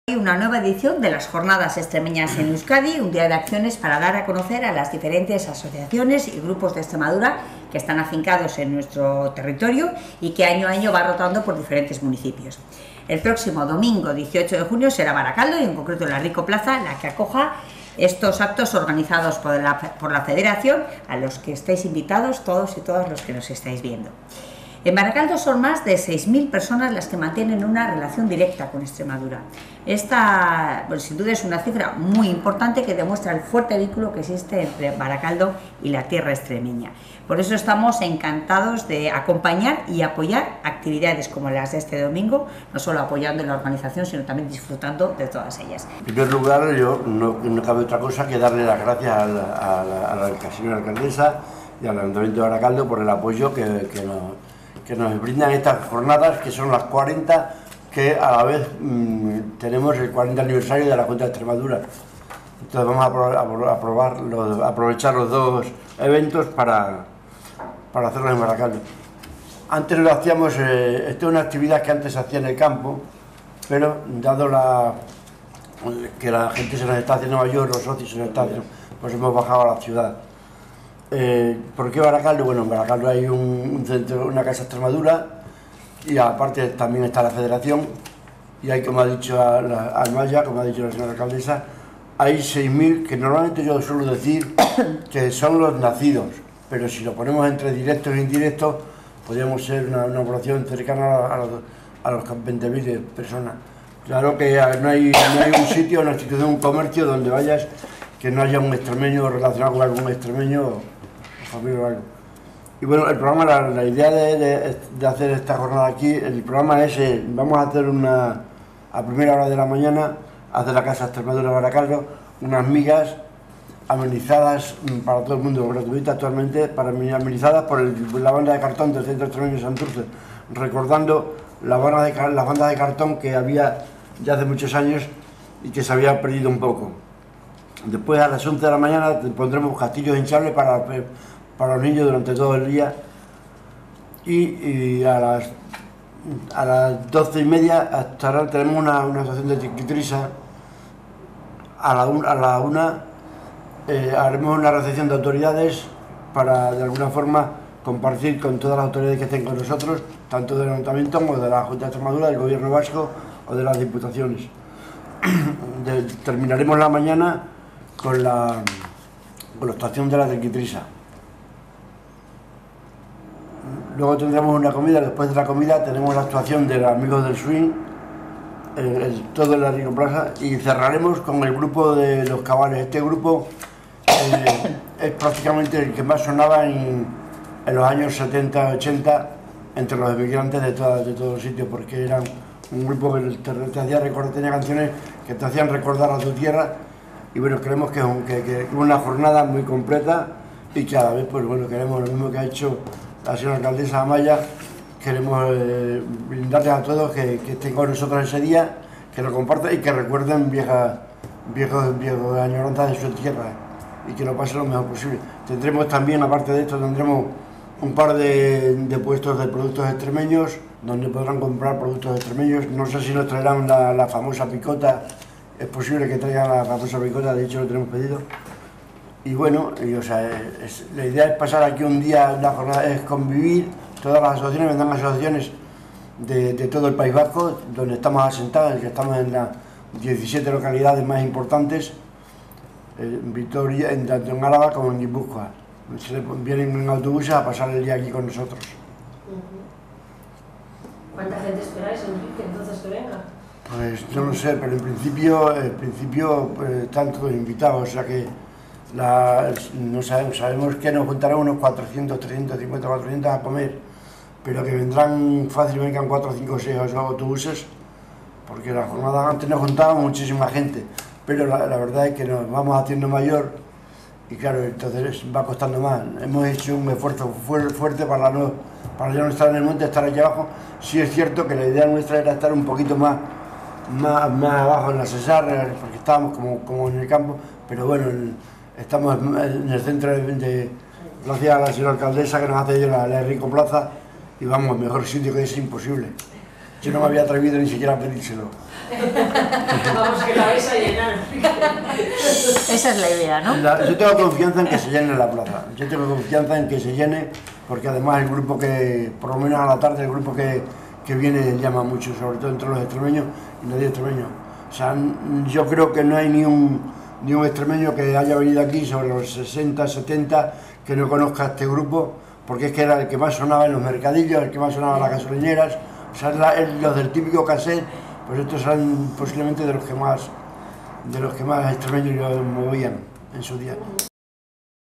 Barakaldo celebrará el domingo las XL Jornadas de Convivencia Extremeña
La Herriko Plaza acogerá actividades infantiles, actuaciones de trikitixa, baile popular con los Amigos del Swing y la actuación del grupo extremeño Los Cabales.
También se celebrarán concursos de aro, rana y corte de jamón.
Se instalarán varios puestos de venta de productos extremeños y un stand de la propia Federación en el que habrá información sobre Extremadura.